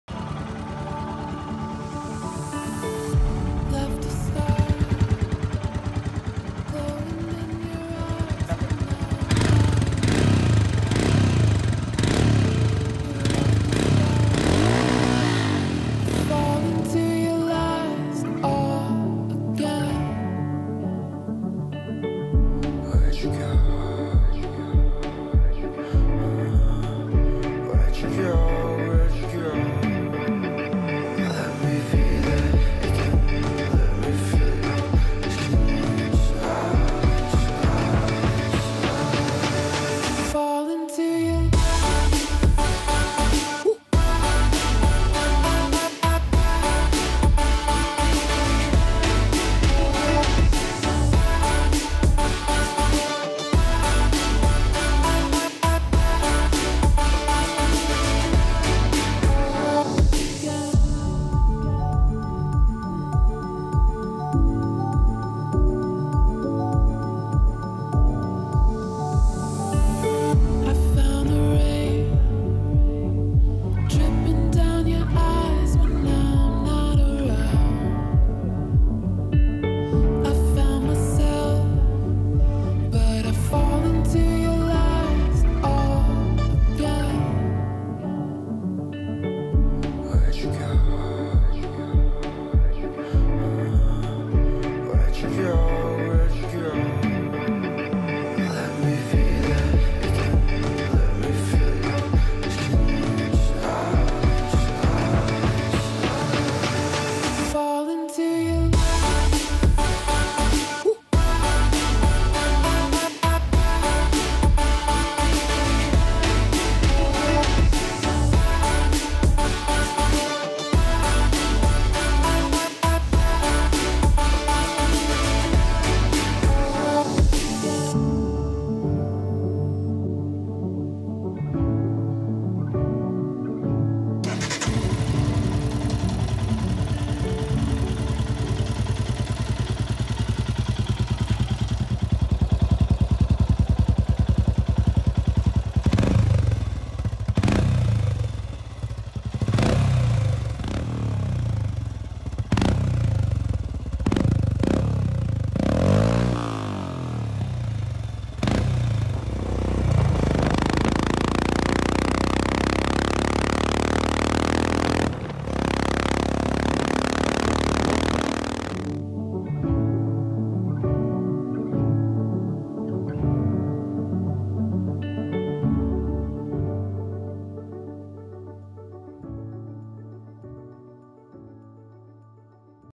UNBOXING SOUND TEST OF RED ROOSTER VERTEX EXHAUST
This video is for all the exhaust lovers. We upgraded a BS6 Himalyan with a Red Rooster slip-on exhaust.
For better experience of the video please use your earphones for better sound quality.

⚠️Note: This exhaust is only for Himalyan⚠️

EDITED BY: Sahil Sancolkar & Nidhish Kalangutkar